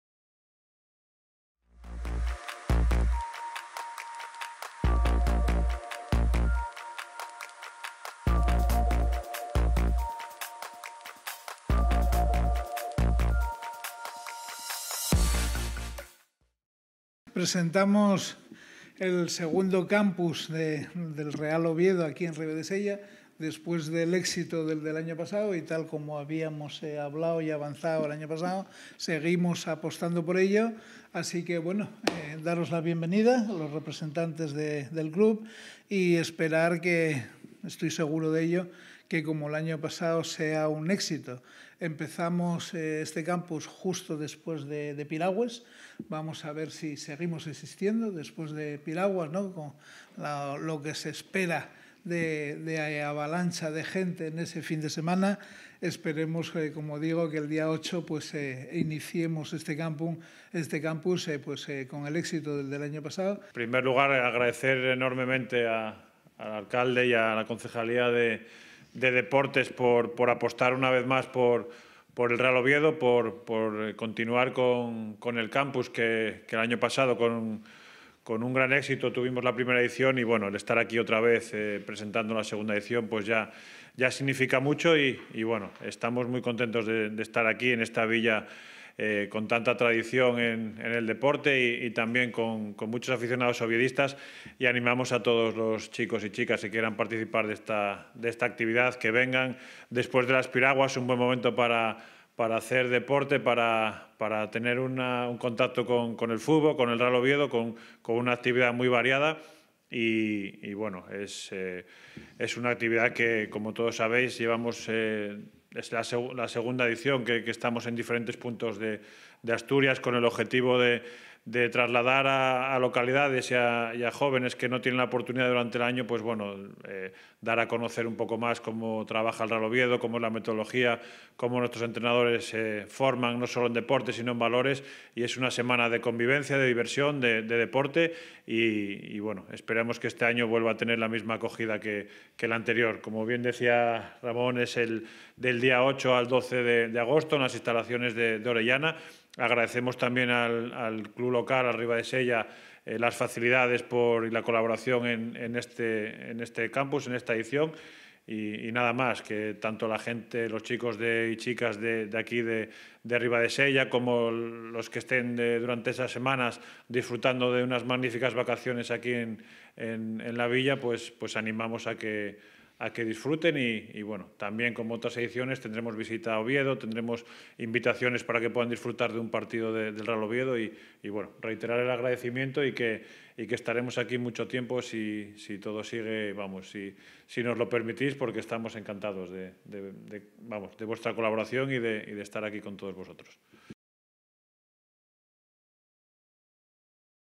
Ribadesella refuerza su apuesta por el Real Oviedo
El Campus de la entidad azul vuelve a la localidad del oriente, en el Campo Municipal de Fútbol de Oreyana del 8 al 12 de agosto